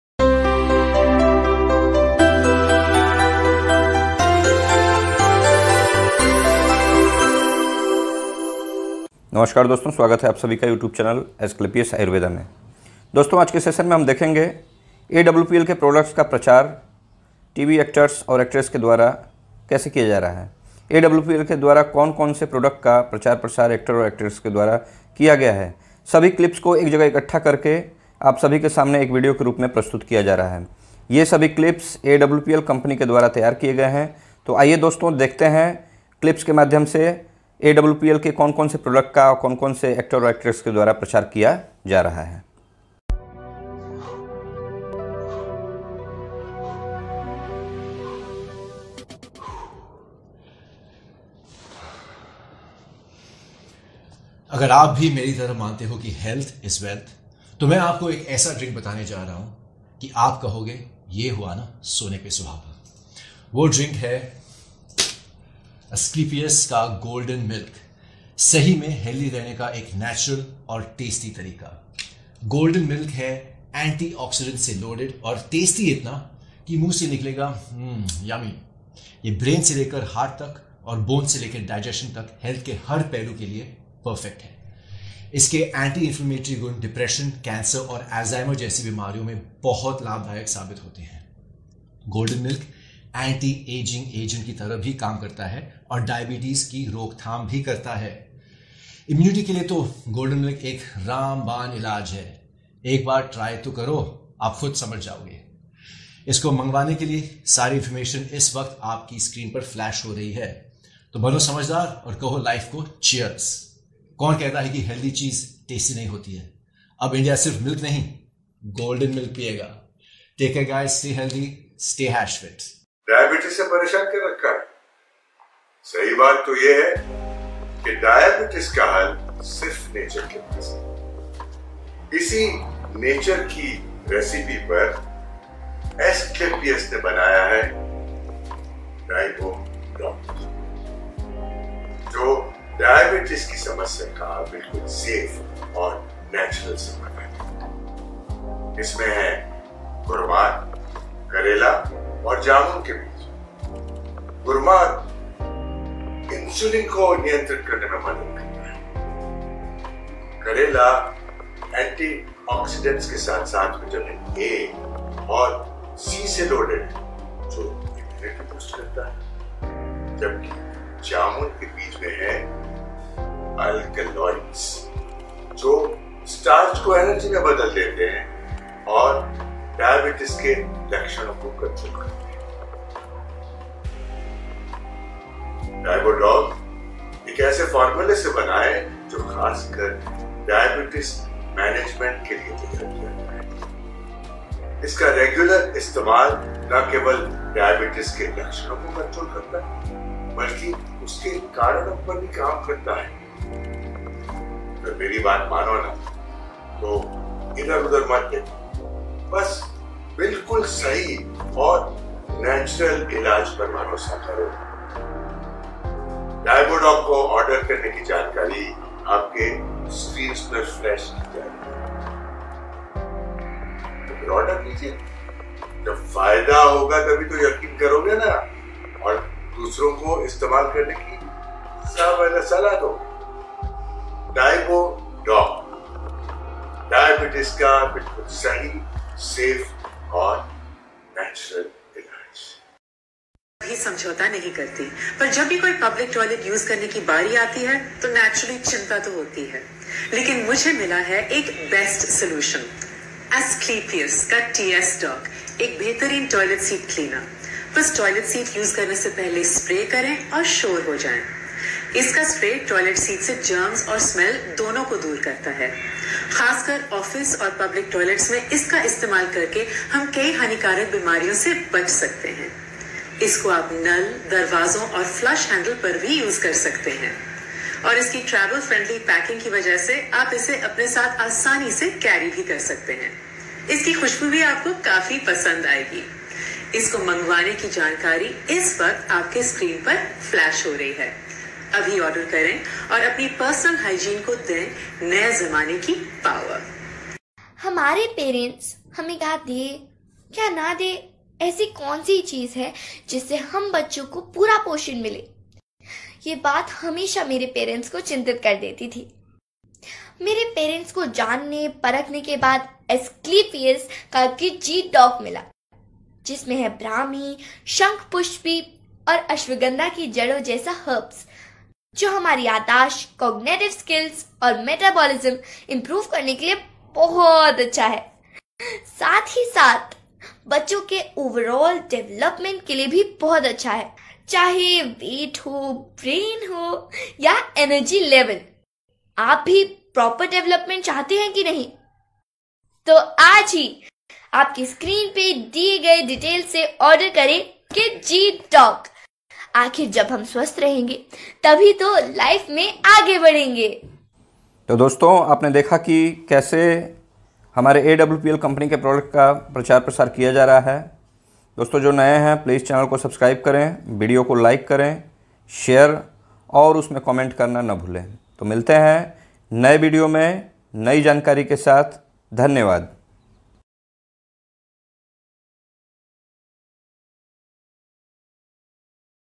AWPL के प्रोडक्ट का प्रचार TV ऐक्टर एवं ऐक्ट्रेस के द्वारा किया गया।
कौन कौन से प्रोडक्ट का प्रचार किया गया

In this video clips are collected by facebook group

सभी क्लिप को फेसबुक से लेकर एक वीडियो में डालकर awpl के फाइटर की सुविधा के लिए बनाया गया है। ये सभी क्लिप्स किसी और के द्वारा रिकॉर्ड किये गए हैं, मैं बस इन्हें एक जगह इकठ्ठा कर इसे एक वीडियो के माध्यम से आप सभी के सामने प्रस्तुत कर रहा हूँ। awpl कंपनी के माध्यम से ये प्रचार करवाया गया है।
Asclepiuswellness, AWPL